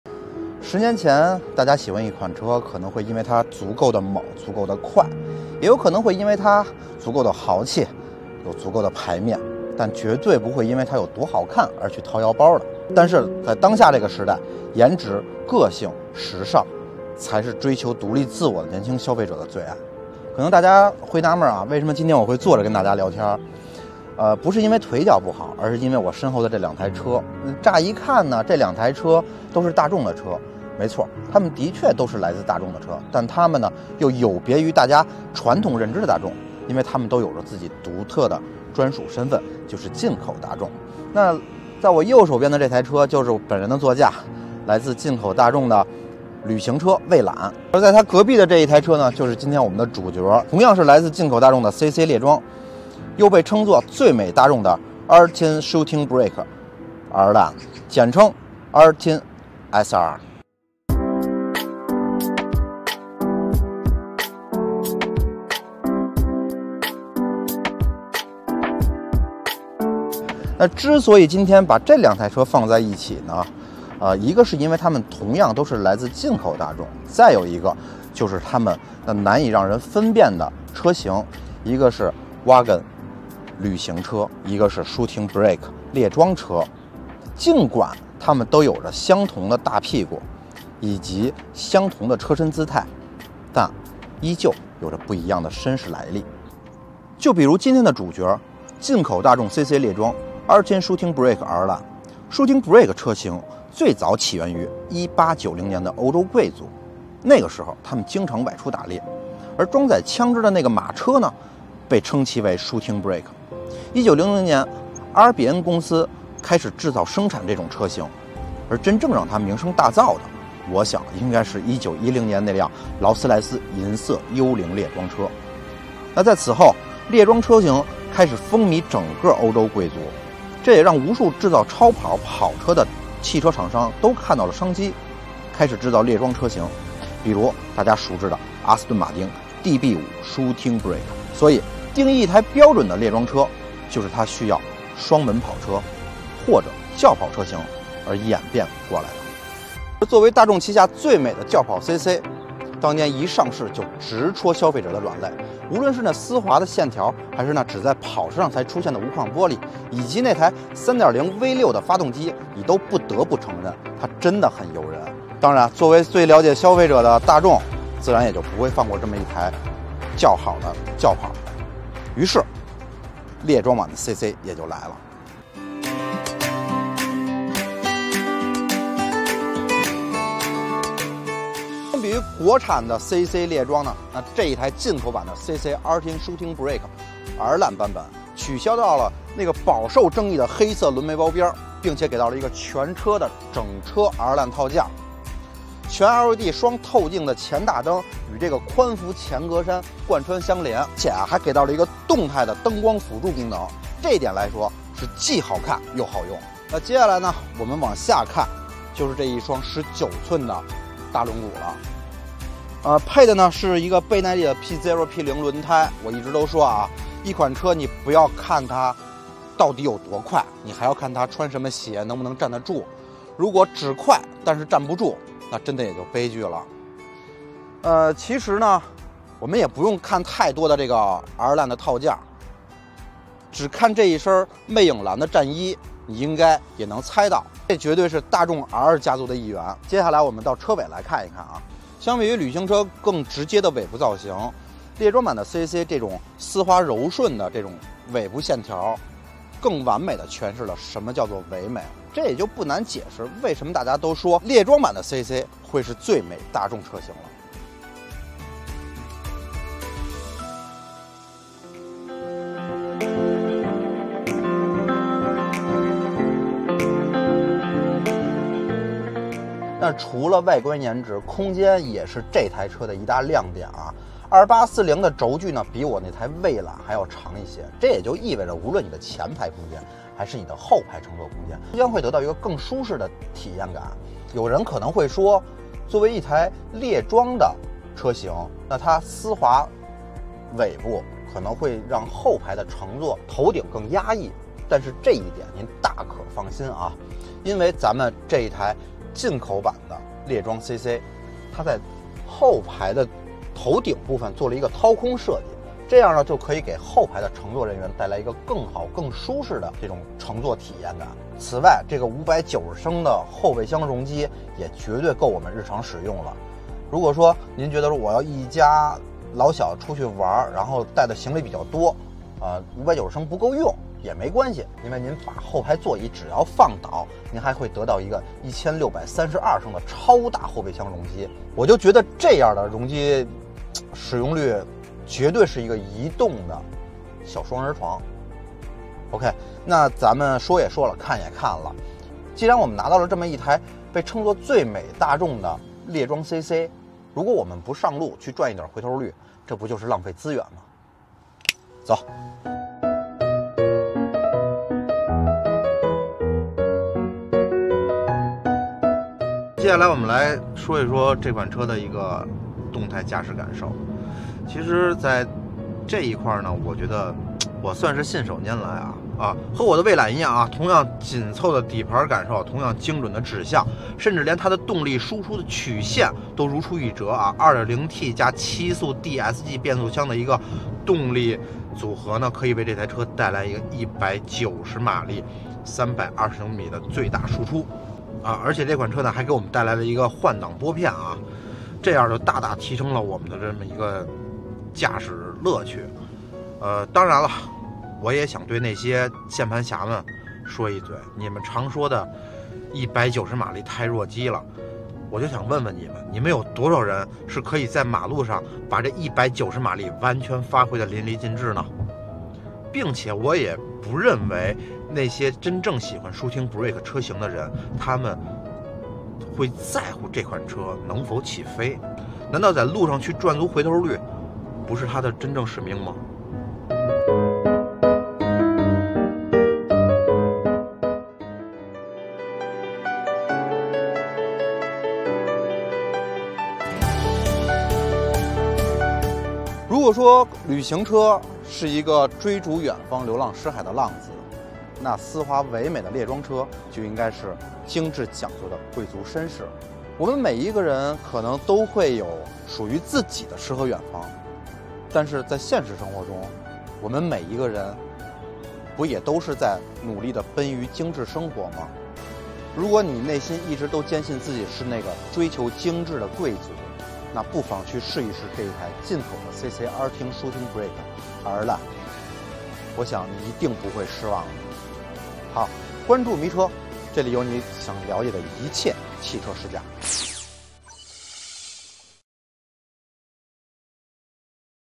浪子VS绅士？进口大众Arteon SR能否勾起你心中的贵族精神
作为大众最美车型的纯进口大众Arteon SR（CC猎装），不仅拥有跑车上才有的无框玻璃，还有着轿跑车型那精美的线条。自1890年开始，shooting brake车型便深受欧洲贵族喜爱，因此让无数跑车制造厂商看到了商机。此后，shooting brake车型便和贵族、绅士牵上了线。此次，试驾进口大众Arteon SR（CC猎装），将为大家详细介绍这款被誉为最美大众的猎装车型。